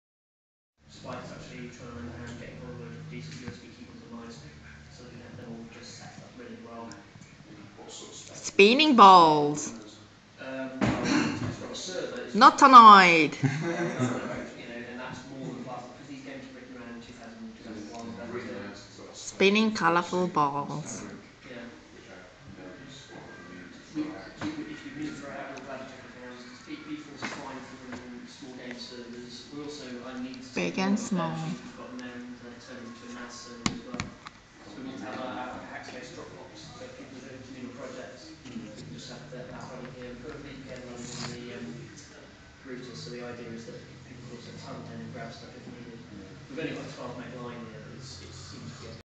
Android multi-touch test: spinning colourful balls
This week at Nottinghack I've been experimenting with multi-touch in Android on a Samsung Galaxy S2 phone.  I only expected it to track two points, but I was surprised to find that it can track up to 10 pointers at once.